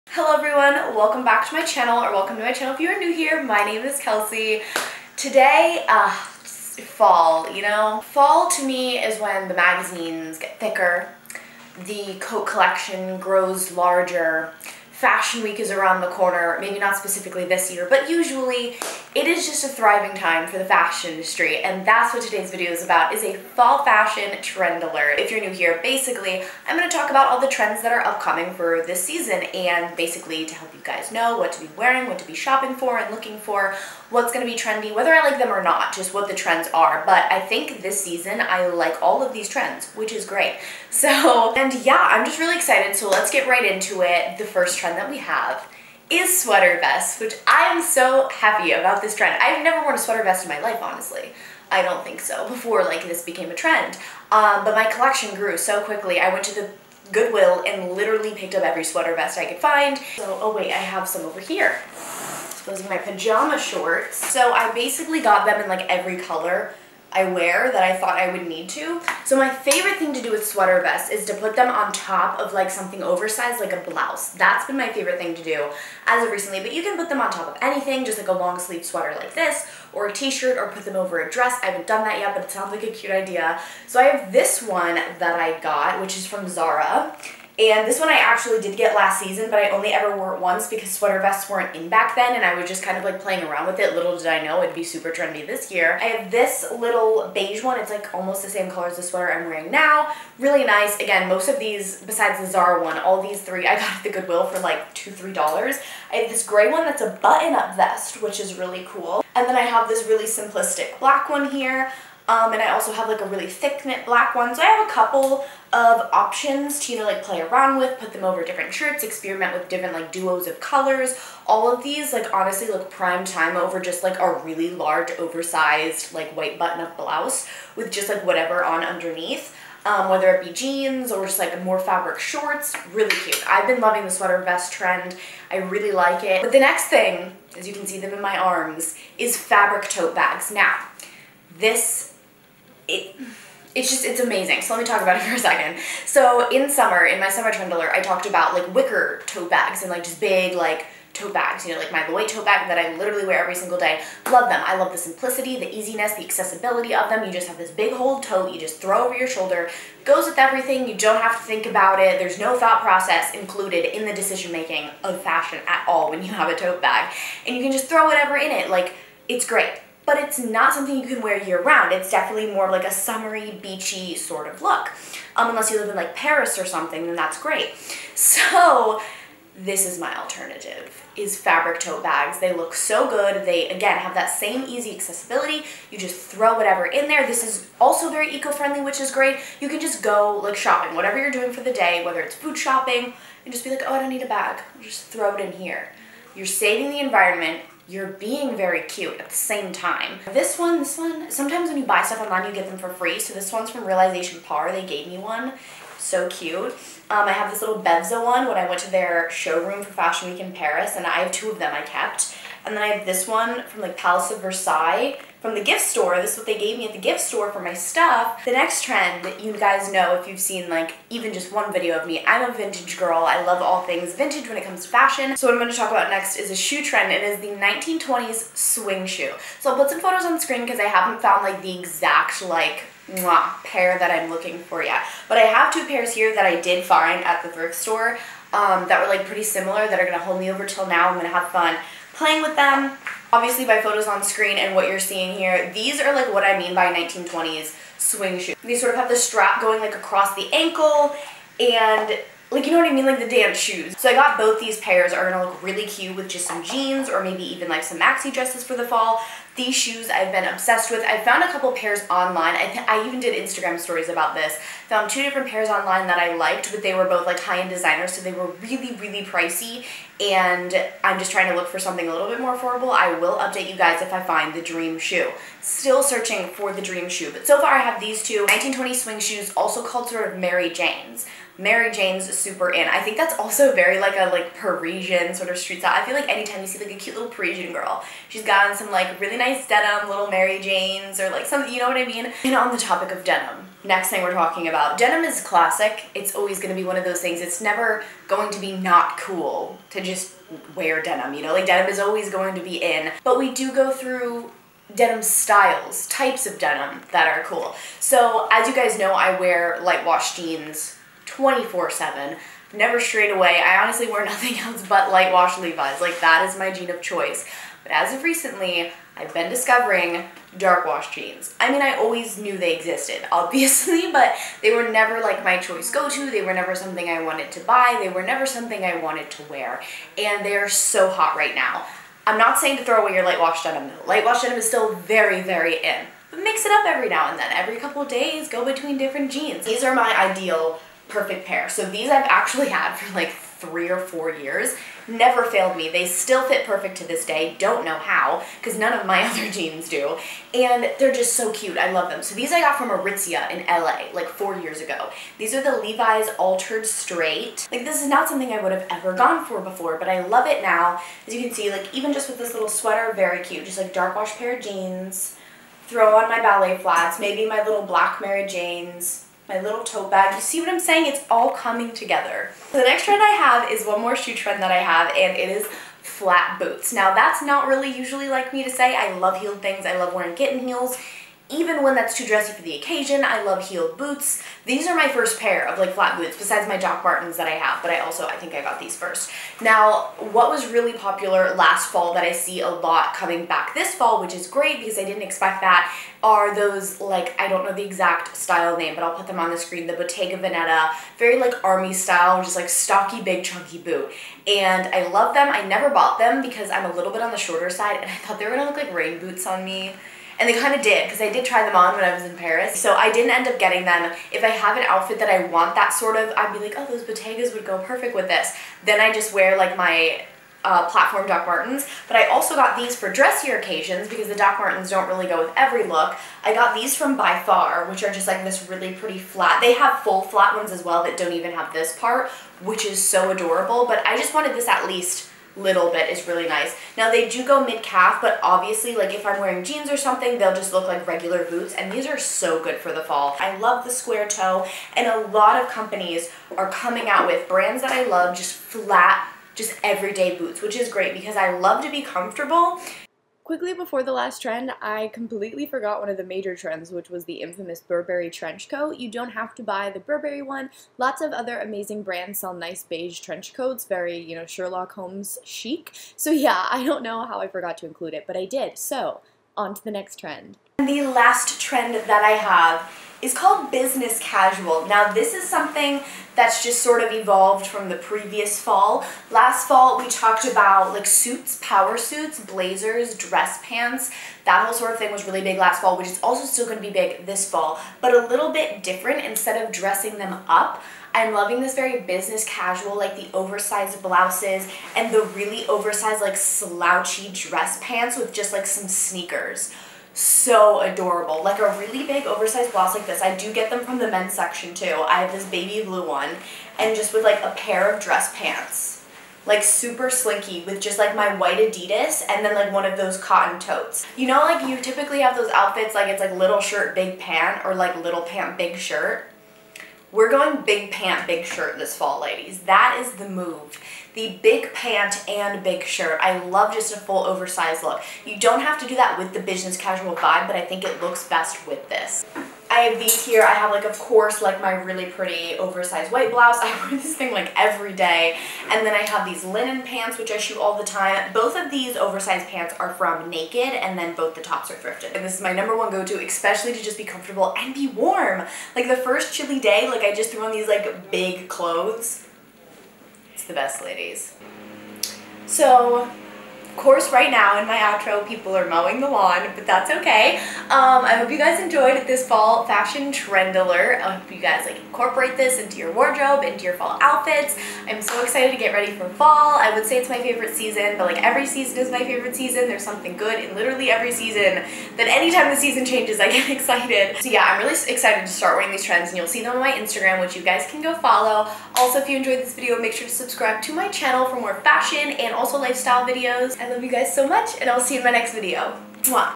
2020 FALL TREND ALERT! What to wear this fall...
These are all of the fashion trends I will be wearing and styling this fall season, see you in my next video... Xx - Kelsey

| Romans 14:19 |

(Use name Kelsey Simone or Kelsey Besanson)